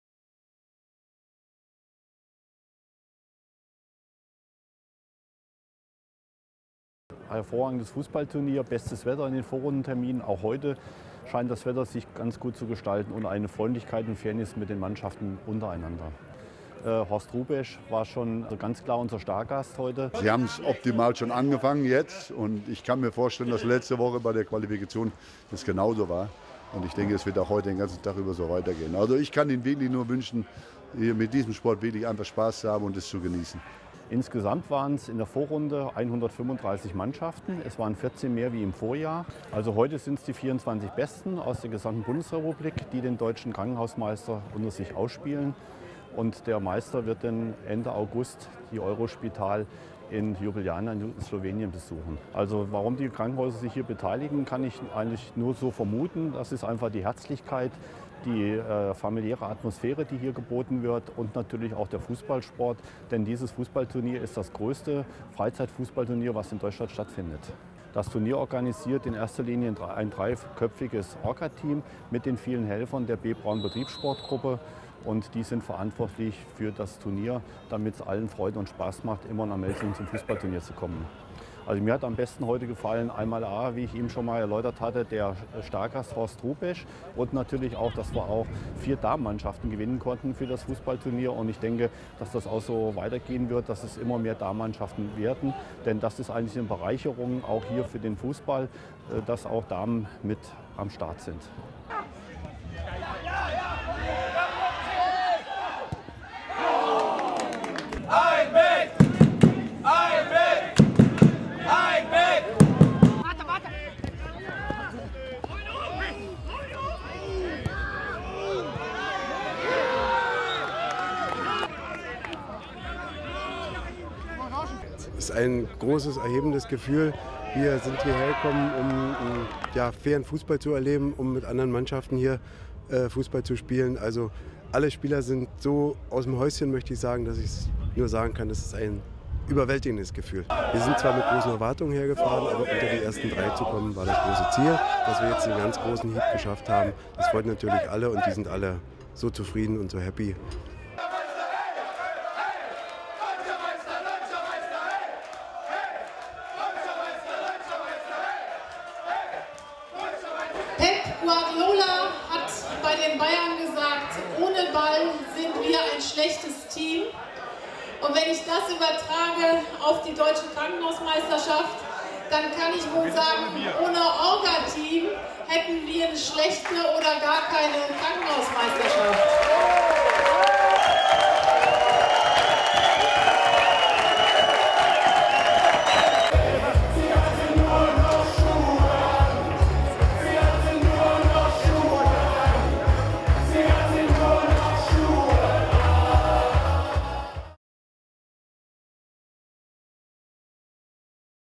31. Deutsche Krankenhausmeisterschaft im Fußball
In Juni 2013 wurde zum 31. Mal die Deutsche Krankenhausmeisterschaft im Fußball in Melsungen ausgetragen. Als Überraschungsgast war Horst Hrubesch gekommen, um die Begegnungen des Finales auszulosen. Das Werner-Forßmann-Krankenhaus Eberswalde konnte am Ende das Turnier für sich entscheiden. Hier einige Videoimpressionen vom Turnier.